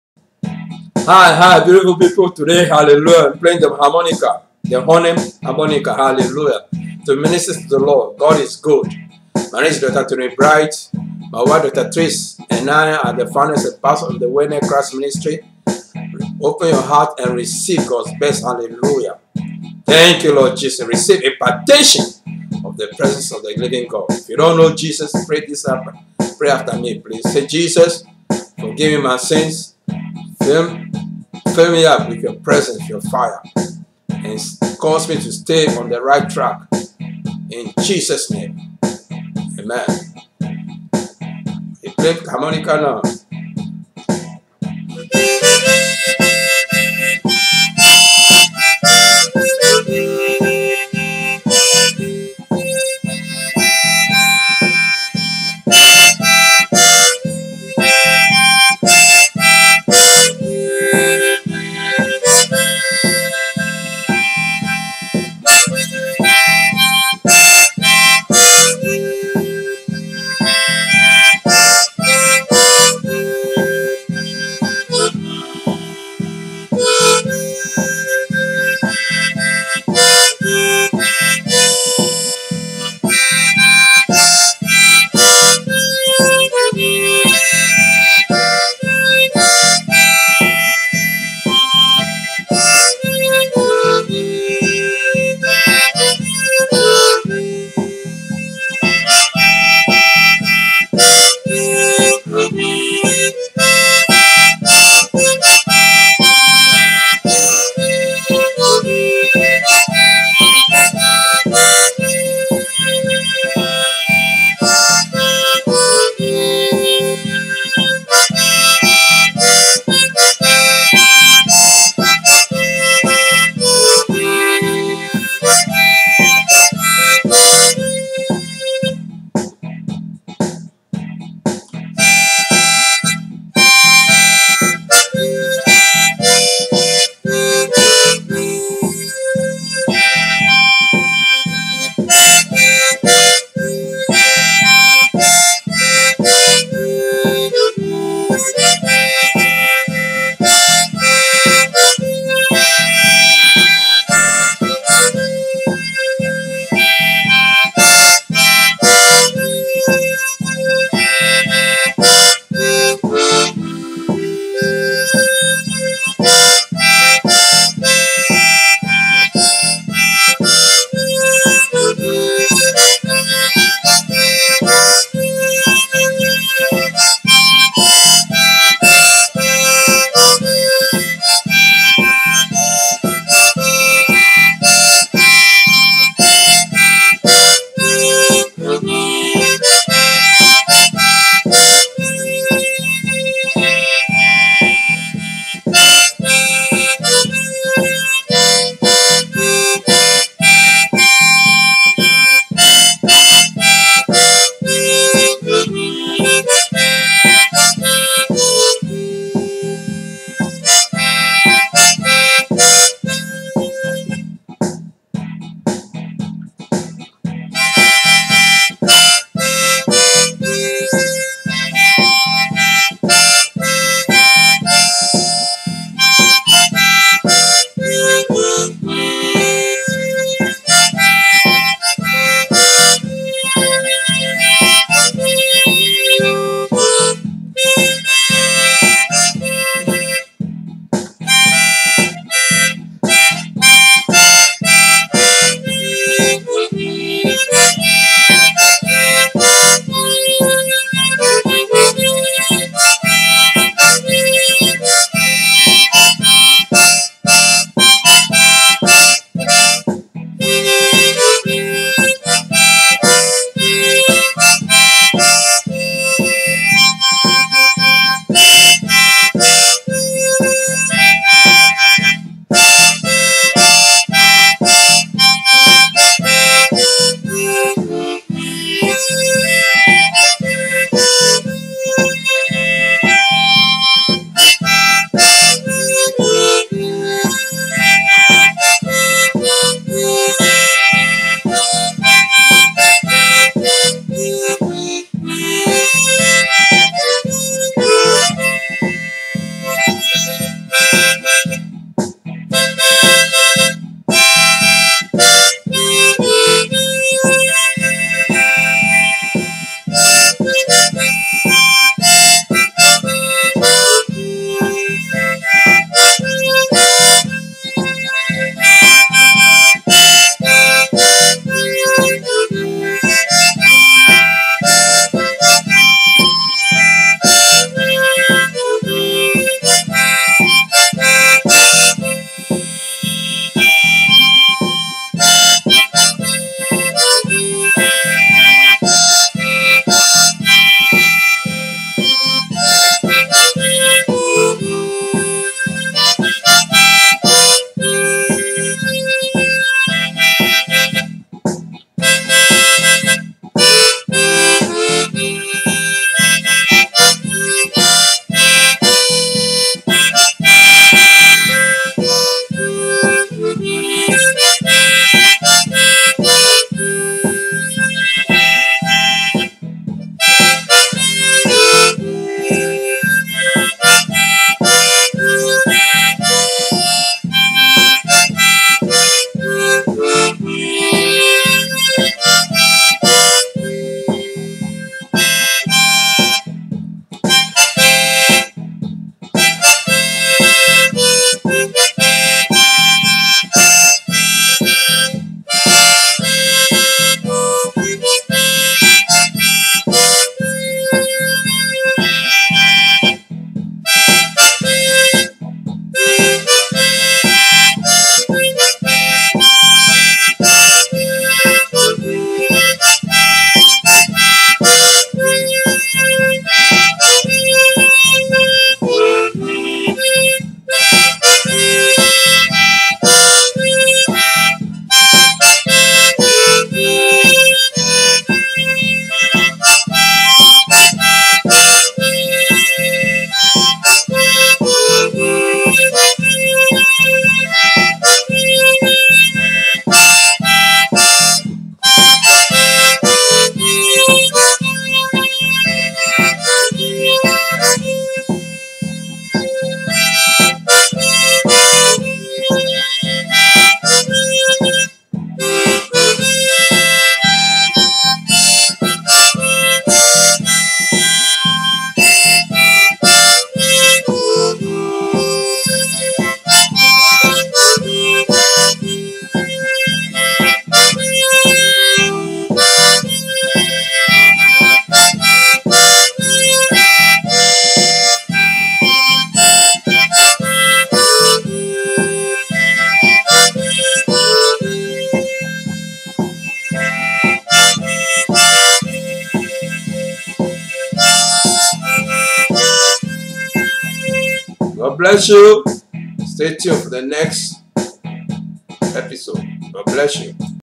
#18 Glory Music-Harmonica6
Dr. Anthony L. Bright Founder & Pastor of Worldnet Christ Ministries is ministering in Music playing the Harmonica to the Lord. The glory, presence, and fire of God was falling. Let this minister to you and be refreshed in the presence of God.